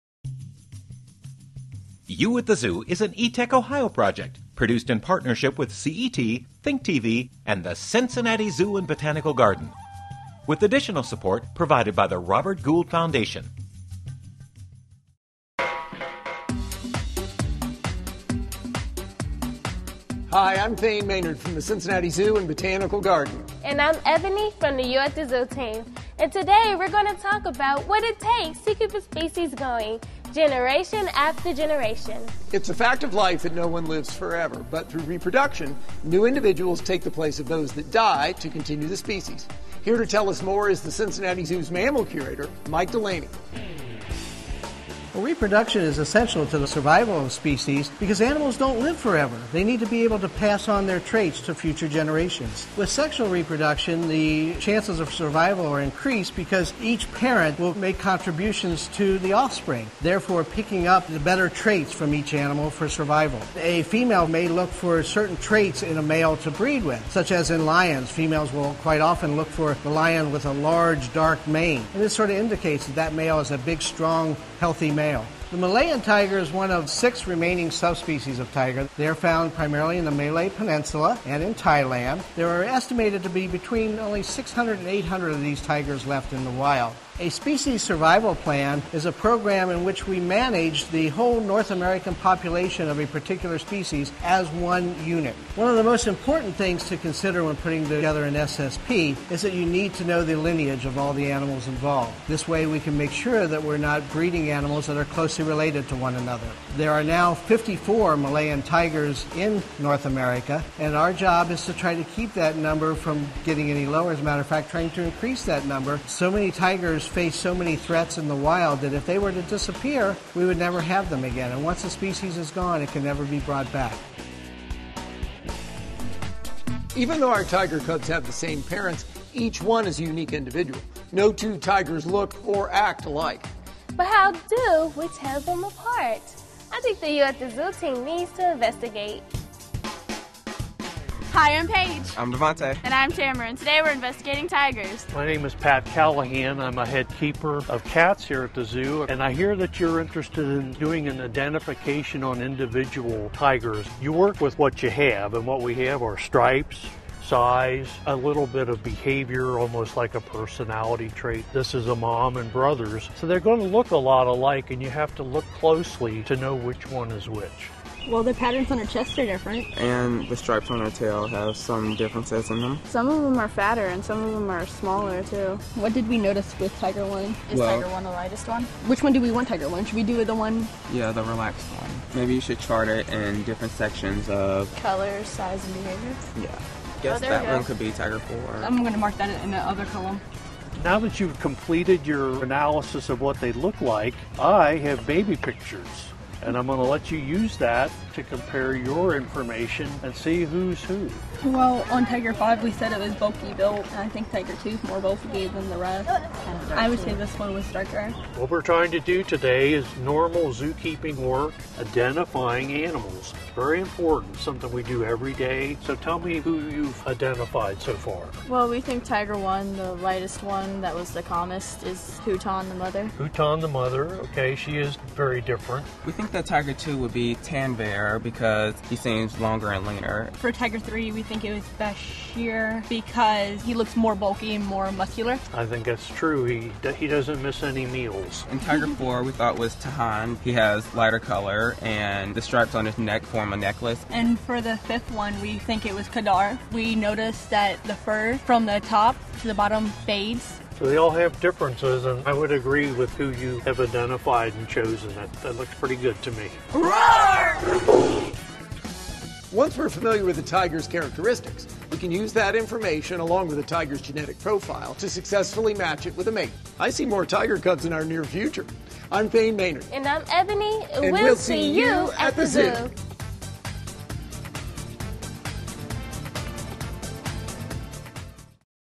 You at the Zoo - The Next Generation
In this video sixth grade students learn about what it takes to keep a species going generation after generation through reproduction. Students also learn about the zoo's Species Survival Plan and the zoo's efforts to save endangered animals from extinction. Then the You at the Zoo team follows the inquiry process to identify tigers by their unique characteristics.